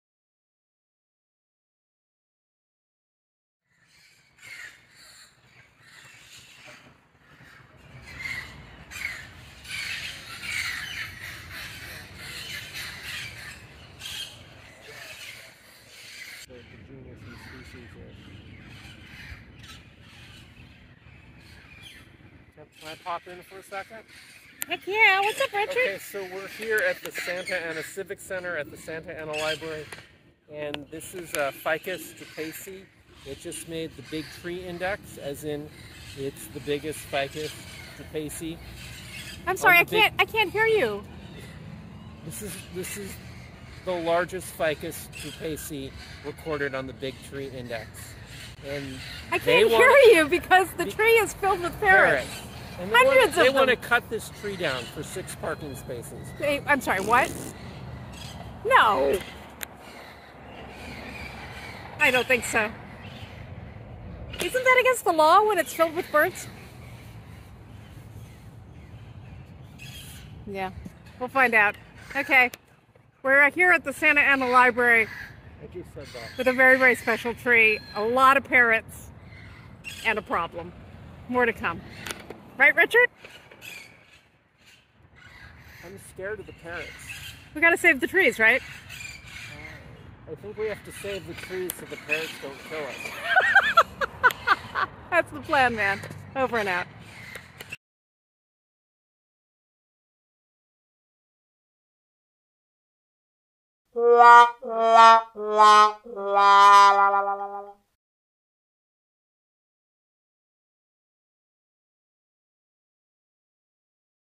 Save the Parrot Tree at Santa Ana Main Library
Please visit to learn more about the threat to one of Southern California's most remarkable trees and how you can help protect it, and keep the parrots from killing Richard.